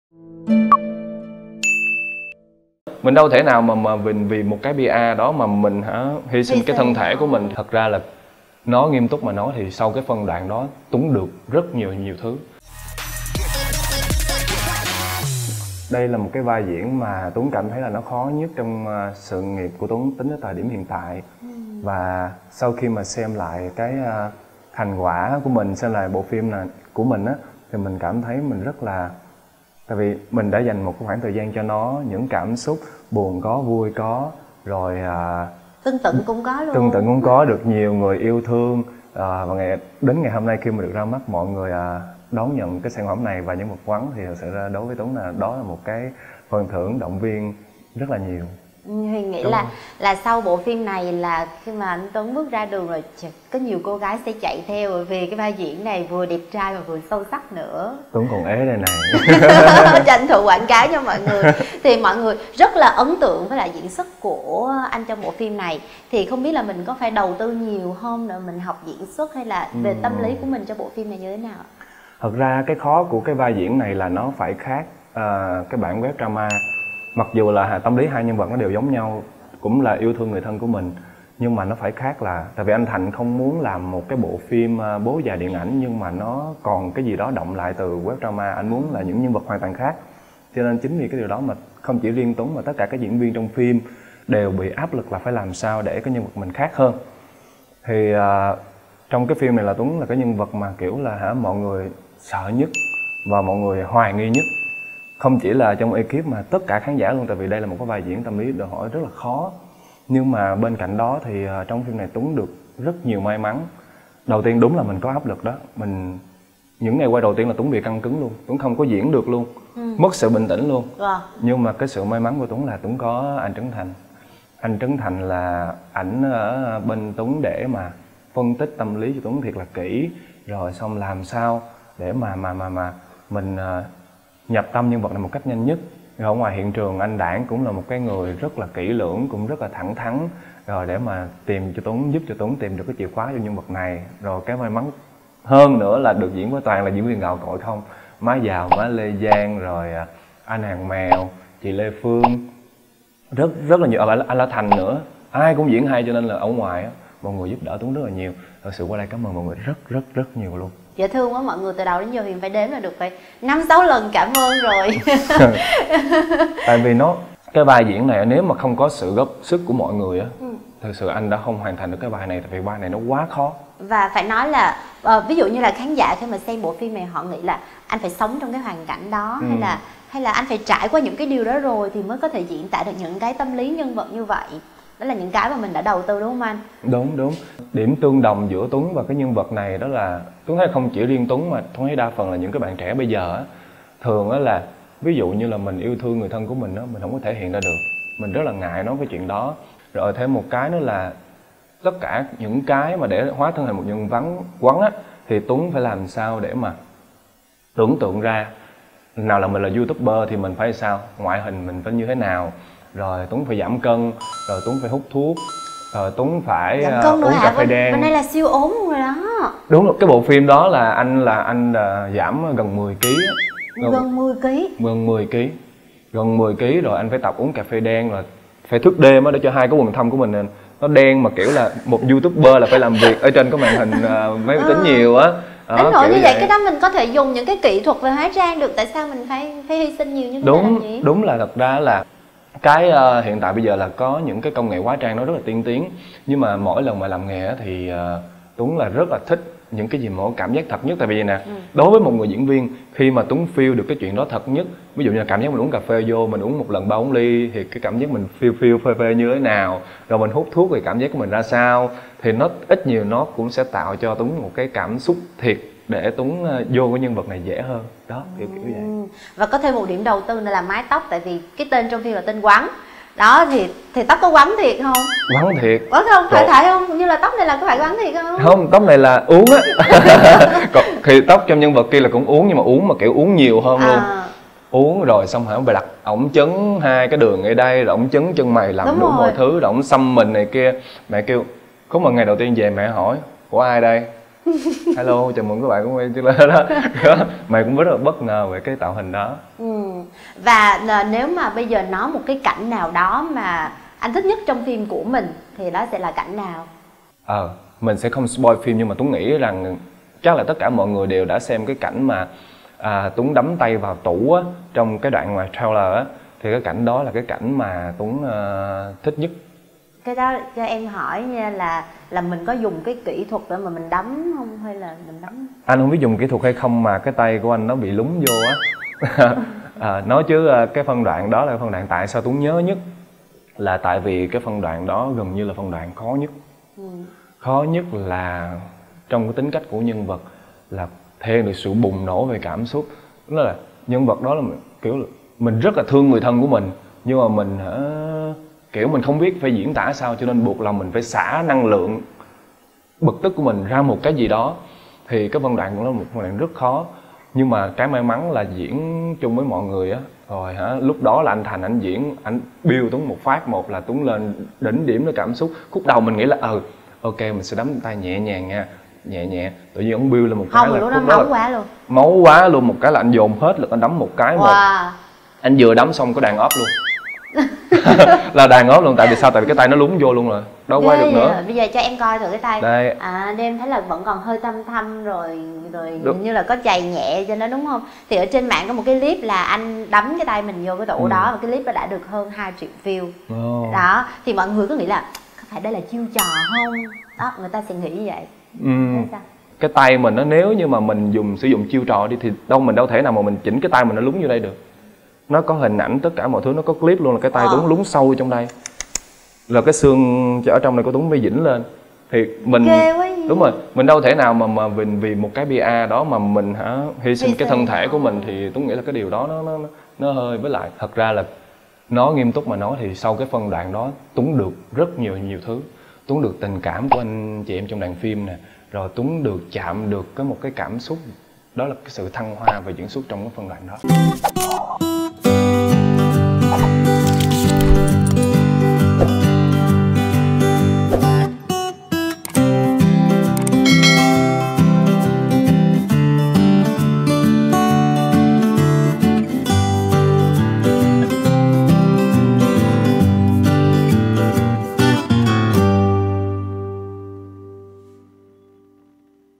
Tuấn Trần “đáp trả” khi bị nói dùng tai nạn để PR phim Bố Già
NÓI ĐI, NGẠI GÌ! | Bị chấn thương tại phim trường, phải điều trị một thời gian, tuy nhiên, Tuấn Trần lại bị nhiều người ác ý cho rằng đây là chiêu PR cho phim Bố già.